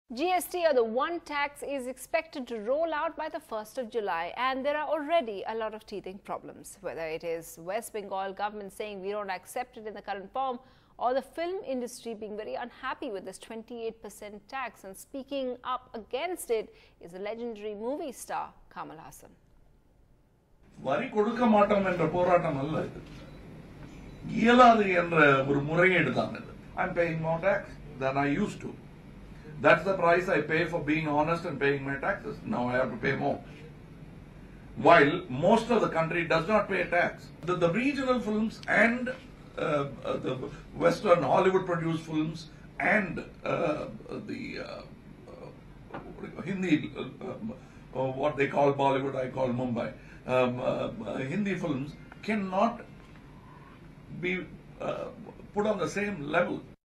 Regional, Hindi Films Cannot Be Put On Same Level: Kamal Haasan On GST
On the increased GST rate, actor Kamal Haasan said that he welcomes GST and one India one tax system, but this rate will ruin regional cinema. "Regional films, Hollywood films and Hindi films cannot be put on the same slab," he said.